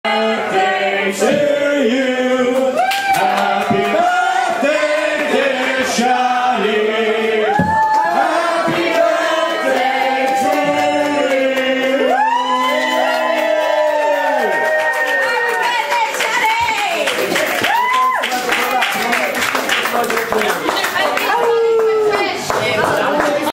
The Donns present Siani Driver with a birthday cake
They're a good bunch.